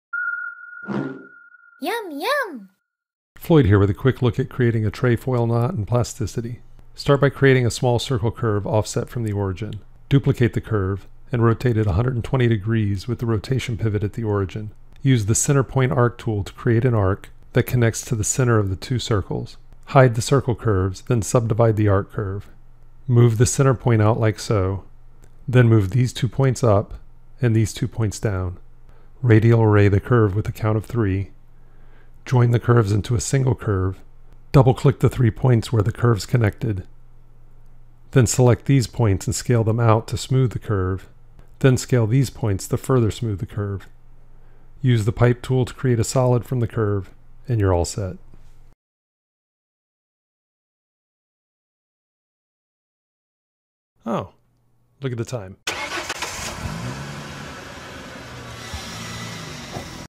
Plasticity | Create a Trefoil Knot
This video takes a quick look at creating a Trefoil Knot in Plasticity.